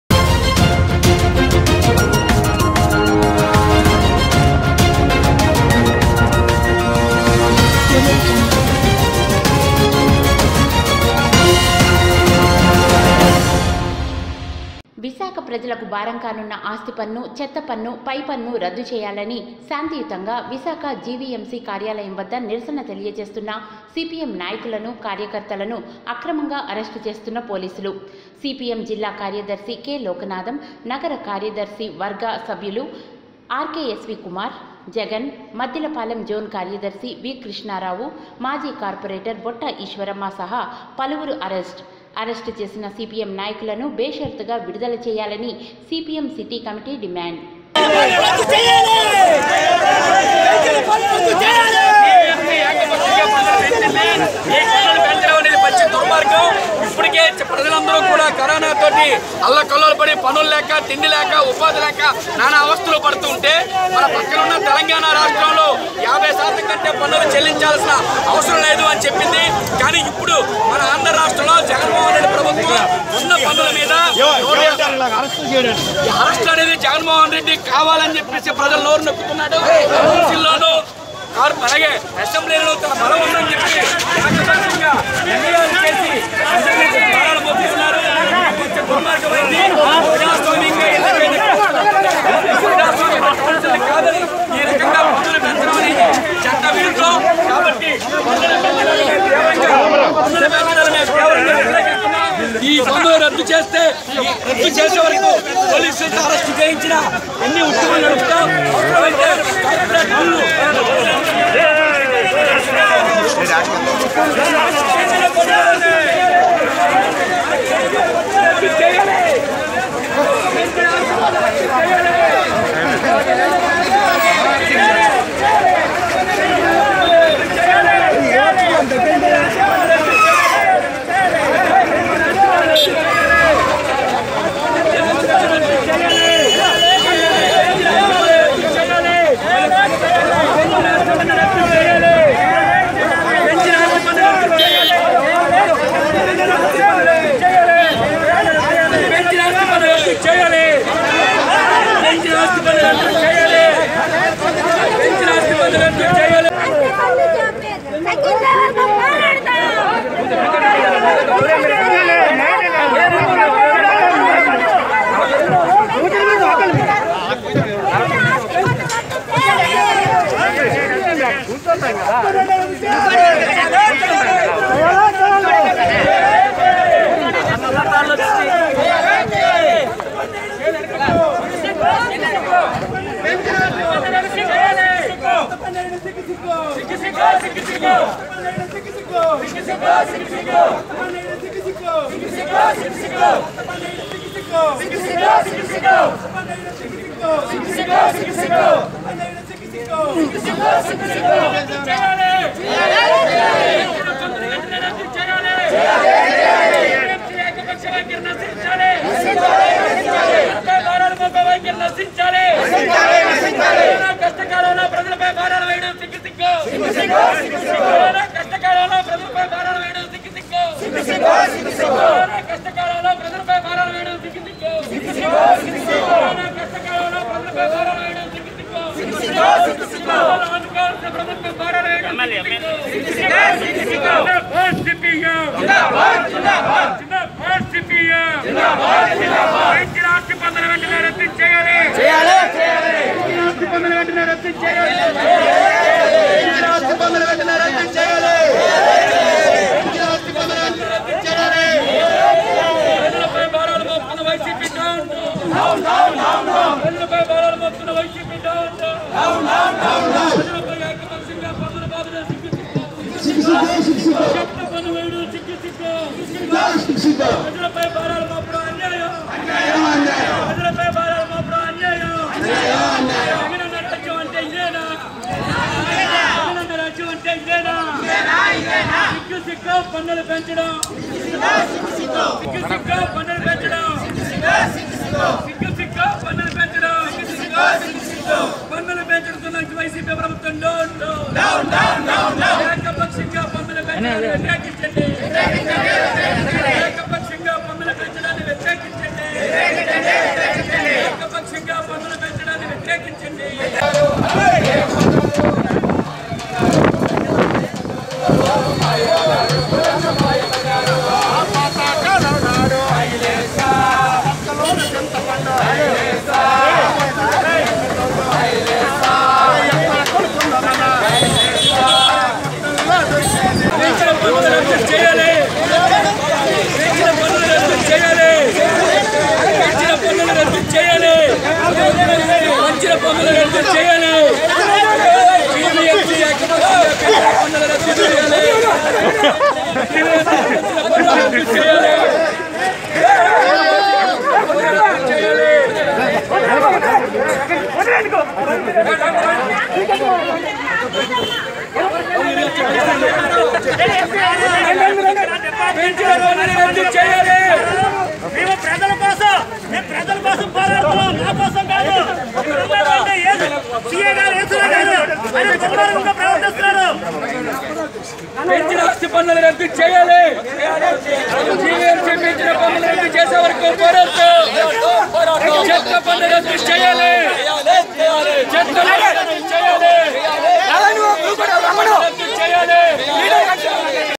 విశాఖ ప్రజలకు భారంకానున్న ఆస్థి పన్ను | sakshitha news
విశాఖ ప్రజలకు భారంకానున్న ఆస్థి పన్ను,చెత్త పై పన్ను రద్దు చేయాలని శాంతియుతంగా విశాఖ జివిఎంసి కార్యాలయం వద్ద  నిరసన తెలియజేస్తున్న సిపిఎం నాయకులను,  కార్యకర్తలను అక్రమంగా అరెస్టు చేస్తున్న పోలీసులు.. సిపిఎం జిల్లా కార్యదర్శి కె.లోకనాథం, నగర కార్యదర్శివర్గ సభ్యులు ఆర్.కె.ఎస్.వి.కుమార్, జగన్, మద్దిలపాలెం జోన్ కార్యదర్శి వి.క్రిష్ణారావు, మాజీ కార్పోరేటర్ బొట్టా ఈశ్వరమ్మ సహా పలువురు అరెస్టు.అరెస్టు చేసిన సిపిఎం నాయకులను బేషరత్తుగా విడుదల చేయాలని సిపిఎం సిటీ కమిటీ డిమాండ్.